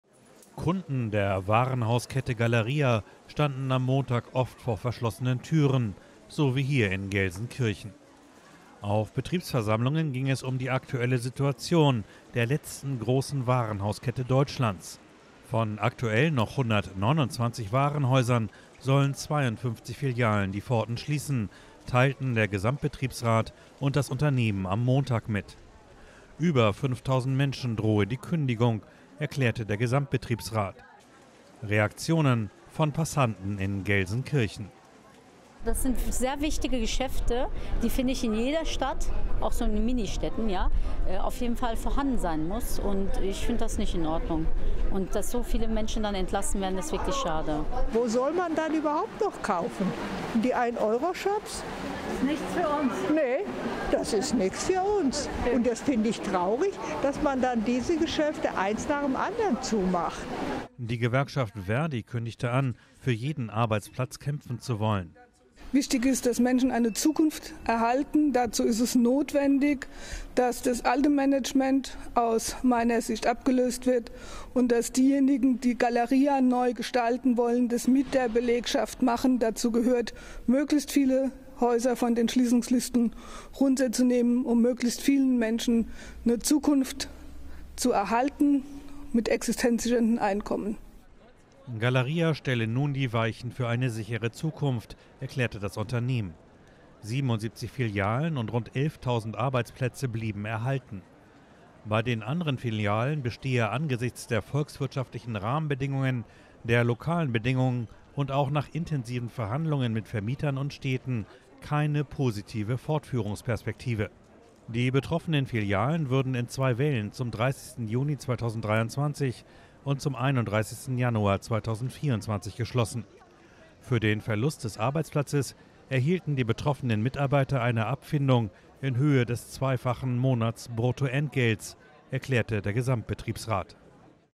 Warenhauskette Galeria Kaufhof schließt 52 Filialen
Galeria Kaufhof befindet sich seit Jahren in einer finanziellen Krise. Von den noch 129 Häusern sollen nun 52 geschlossen werden. Etwa 4.300 Beschäftigte sind betroffen.

Bildrechte Thumbnail: Michele Tantussi/Reuters